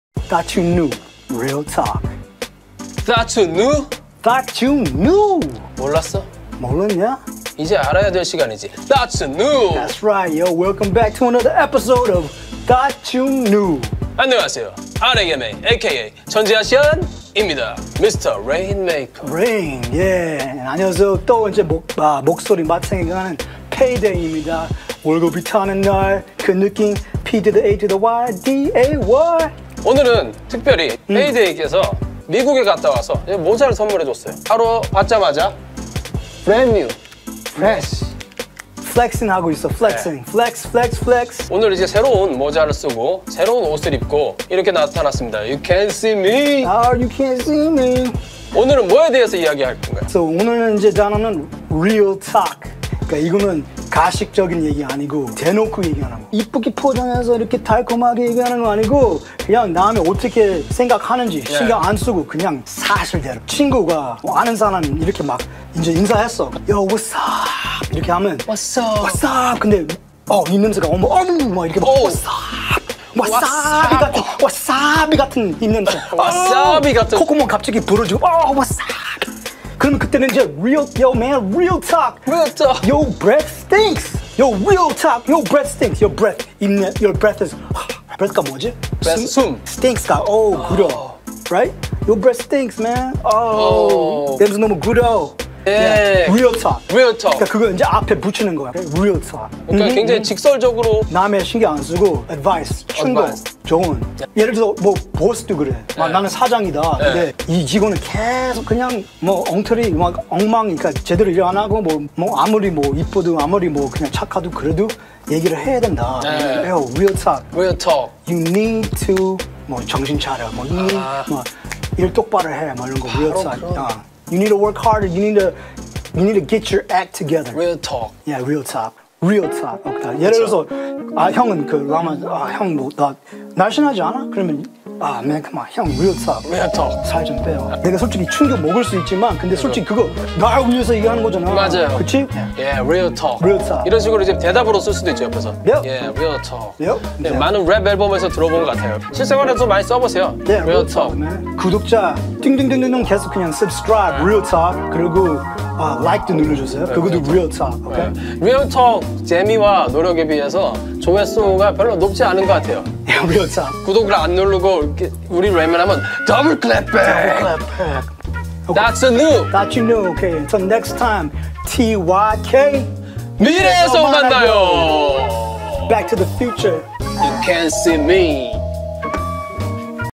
[따츄뉴 제6화] REAL TALK 솔직히 뻥안치고 최고로 유익하다 - 서울문화재단 청년 크리에이터
솔직히 뻥안치고
정말  재밌고 유익합니다!
노력과 재미에 비해 언더레이티드되고 있는 따츄뉴~

현재 한국에서 힙합문화는 메인스트림이 되었고 많은 젊은이들은 랩구절을 암송하며 여러 표현들을 따라합니다.
하지만 모두 올바른 뜻과 유래를 알고 제대로 사용하는 것일까요?
과거 KBS 해피투게더에서 이효리와 함께 영어를 배워보는 유익한 코너를 진행했던 한국힙합 1세대 레전드.
유명힙합그룹 DNS의 멤버였던 ‘실력’ 그가 PayDay라는 이름으로 돌아왔습니다.
그와 함께 유익한 표현들을 익혀보며 힙합문화와 가까워지는 시간을 가져봅시다

제작:  서울문화재단 청년 문화 크리에이터 유희재